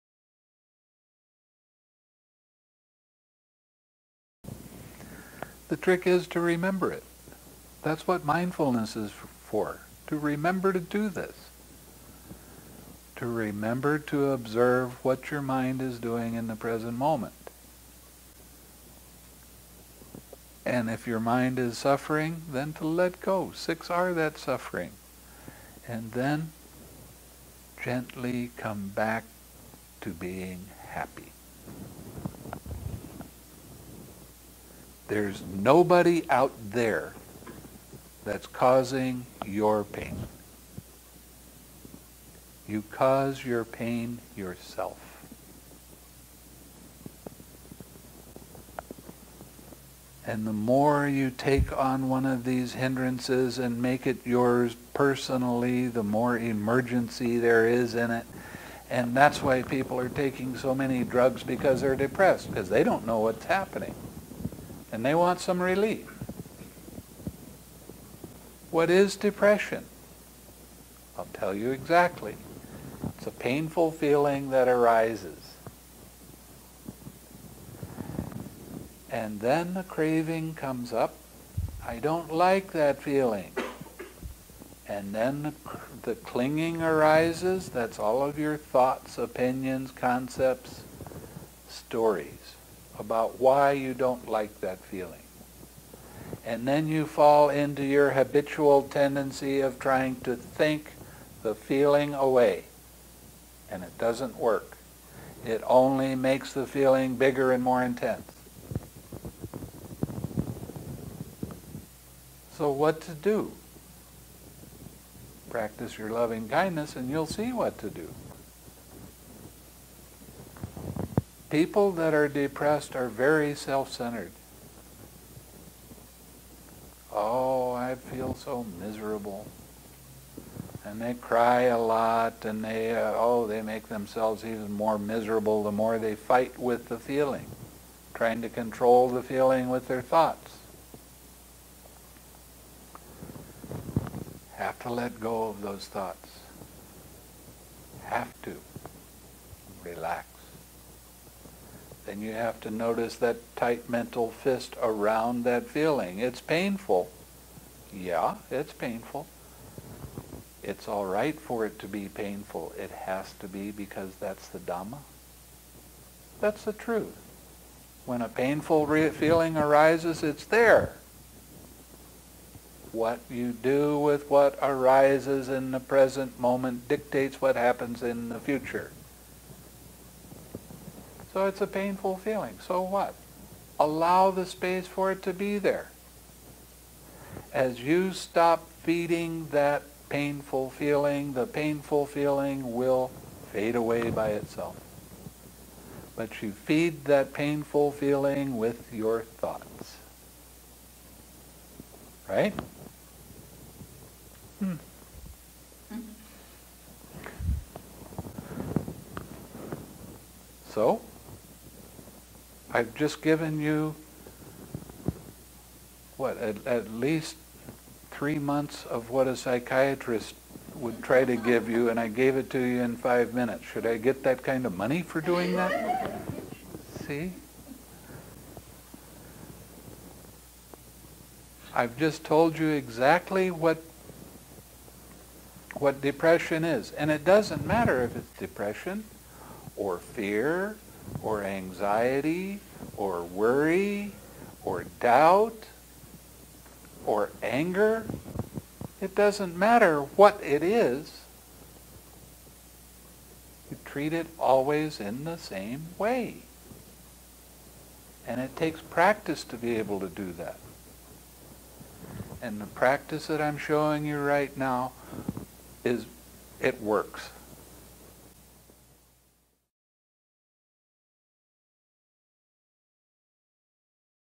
Talk on Depression (With Chinese Subtitles) Bhante Vimalaramsi
Bhante Vimalaramsi talks about causes and cures for Depression

Where: DSMC
Talk by Bhante Vimalaramsi
Abbot - Dhamma Sukha Meditation Center